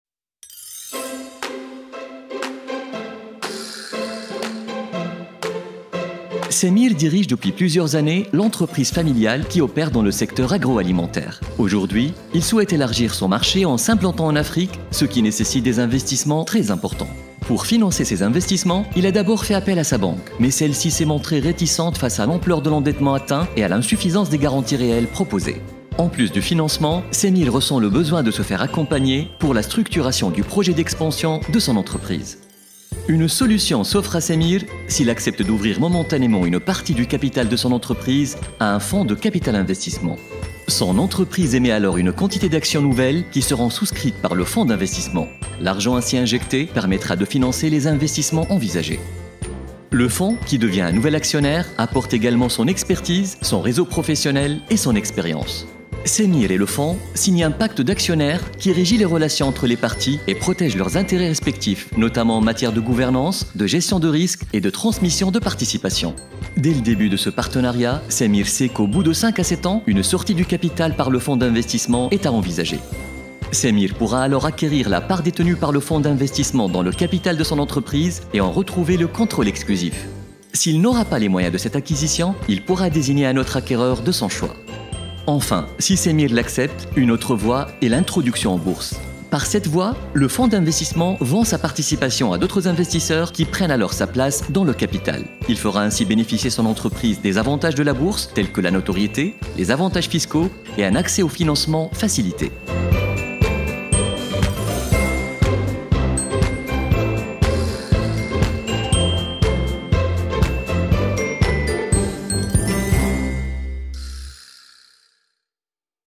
Le capital investissement, une alternative au financement bancaire
Samir, dirigeant d'une entreprise familiale, fait appel au capital investissement pour se doter des moyens de ses ambitions.